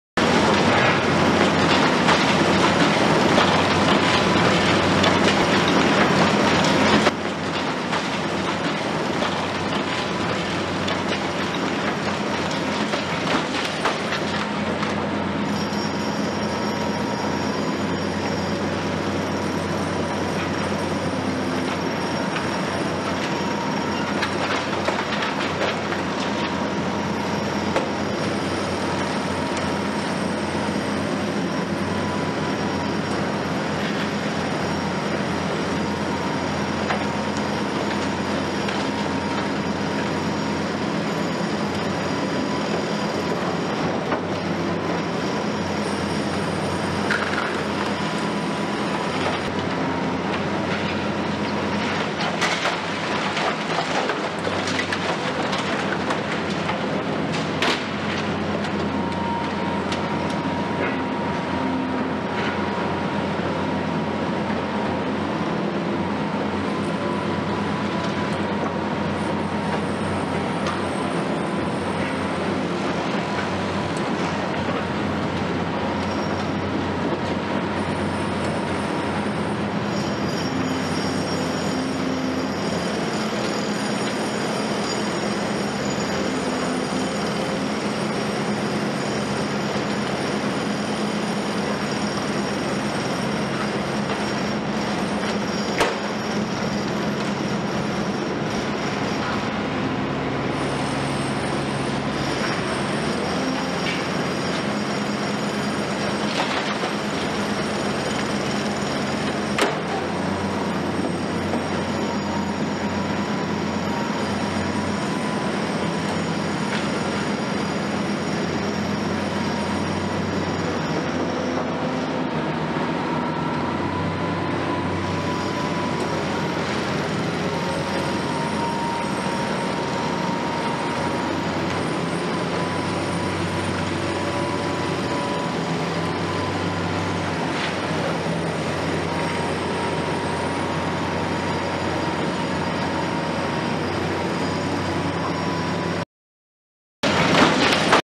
großer Bagger Volvo EC700 und Zaxis 470 beim Abriss
Baustelle Bastionstraße Düsseldorf hier mach Laarakkers den Abriss der alten Verdi Niederlassung. Der Abriss ist extrem kompliziert, weil das Gabäuge direkt an der Straße steht. Mit einer Kombination von Containern und einem Gerüst wird für Sicherheit gesorgt.